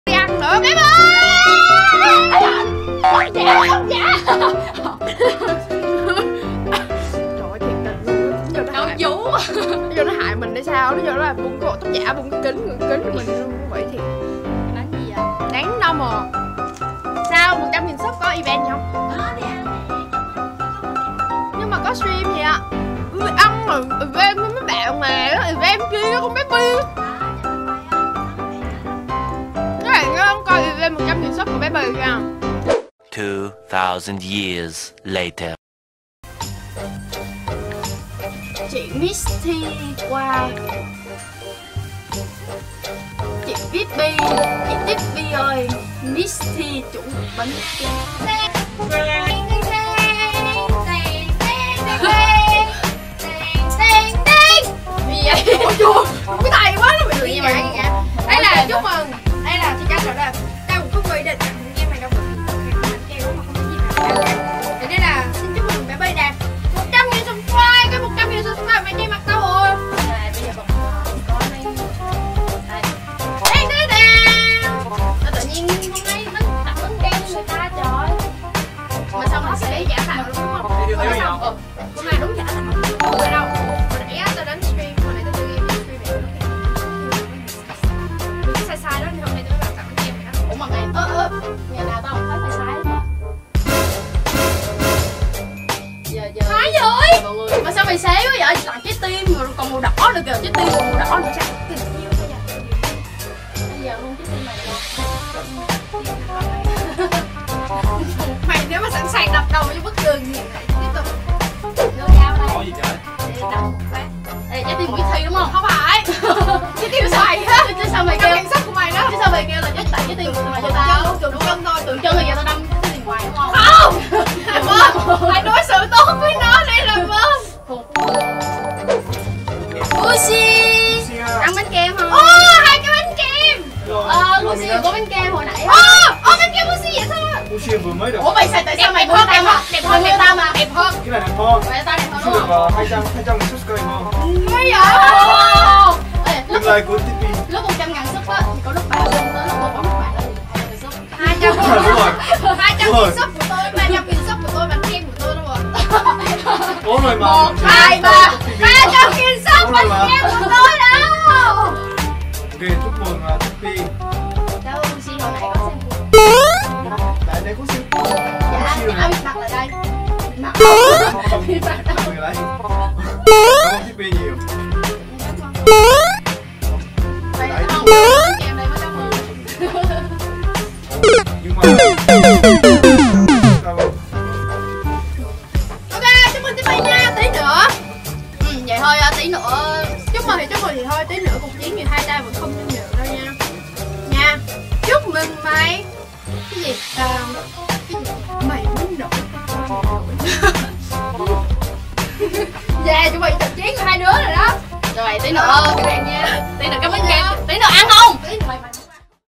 MISTHY - MINAS - WOOSSI ĐỘT KÍCH PHÒNG TIPPY TẶNG BÁNH KEM CHÚC MỪNG 100.000 SUB
Đây là món quà mà Misthy giành tặng cho Tippy nhân dịp đạt 100.000 Subs
Cùng đó game thủ nói tiếng liên minh huyền thoại Việt Nam đó là Minas cũng tham gia chúc mừng Tippy.